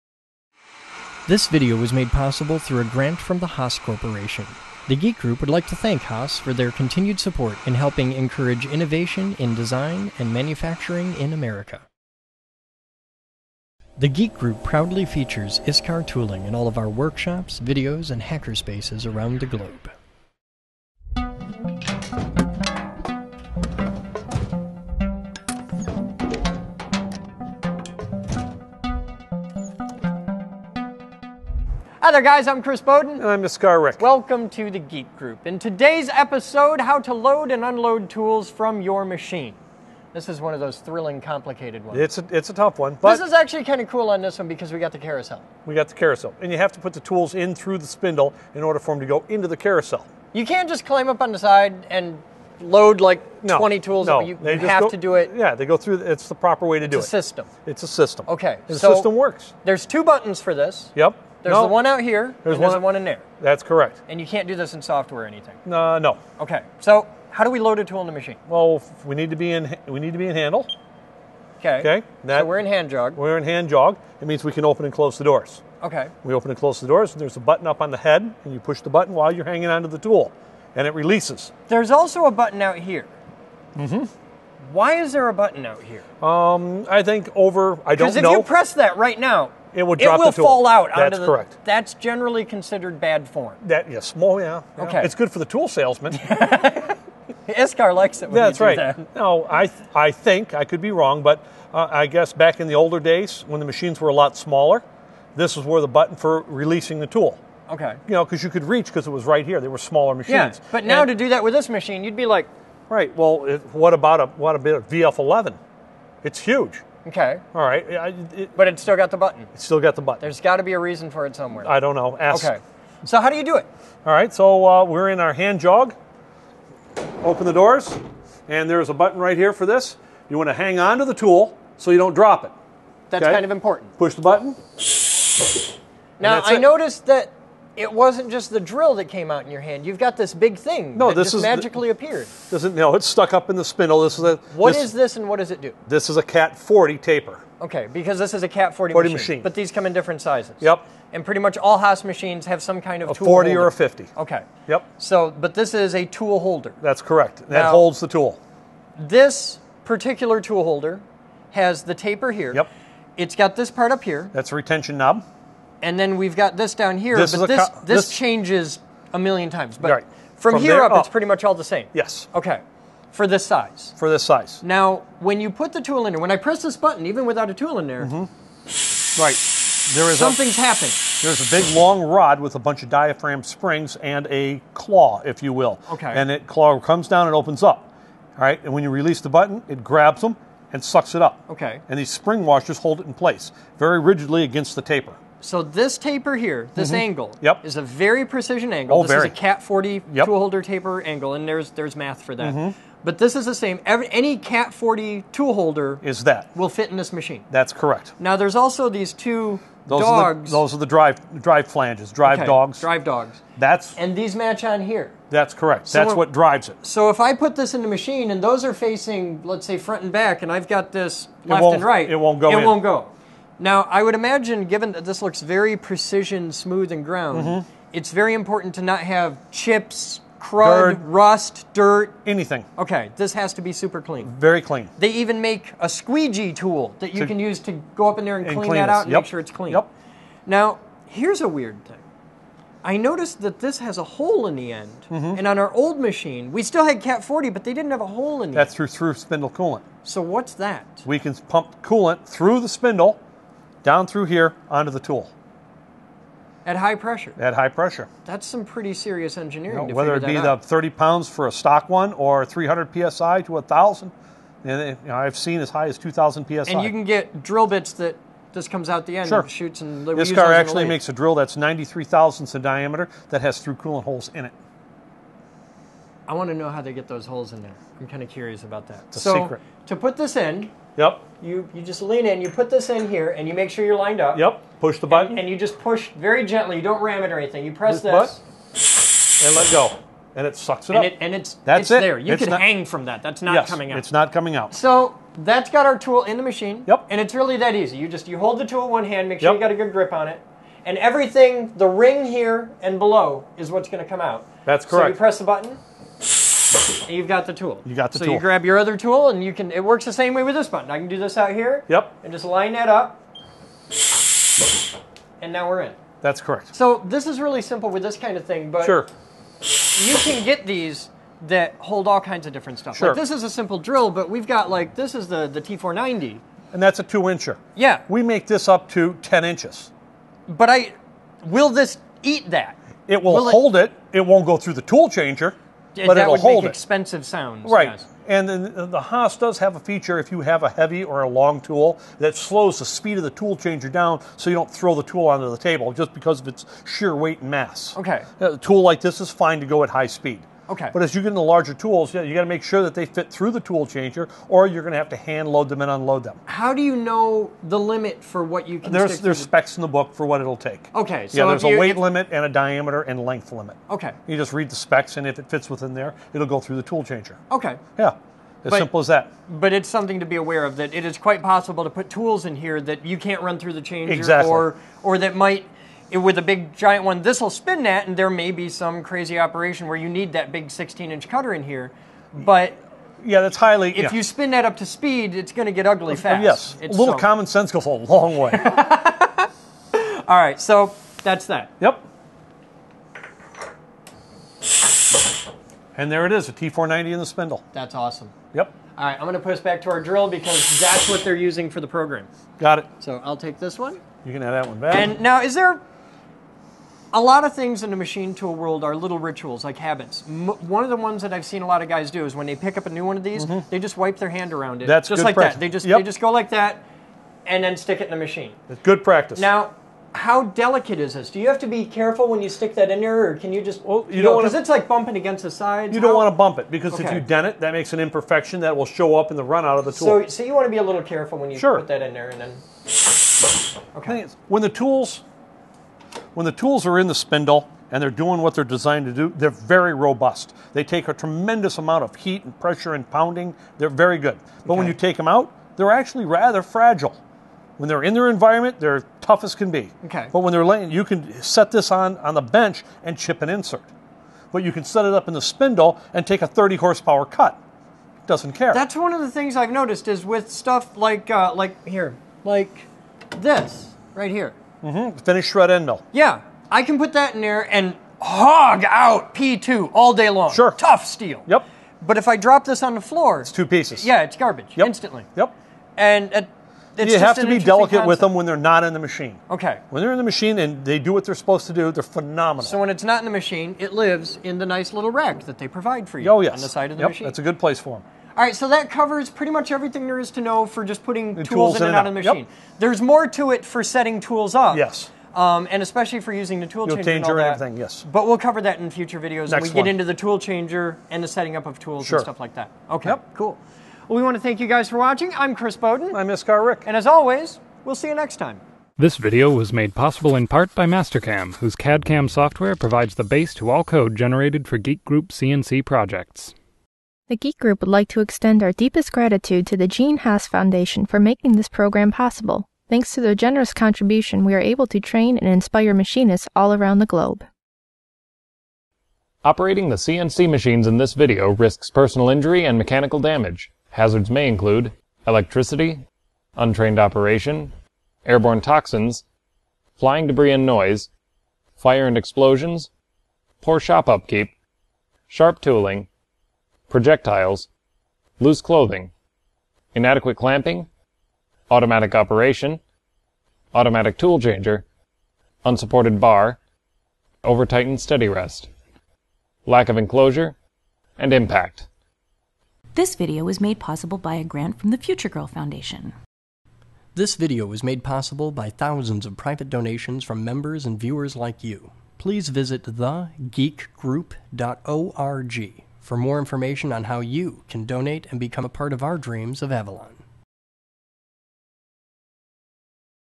Loading Tools Into the Spindle: CNC Training Series
- Before you can cut, you have to get your tool into the machine! Chris and Rick cover the best practices of inserting a tool into the spindle. Watch to see what the machine actually does during the process, and also pick up a few tips on tool care!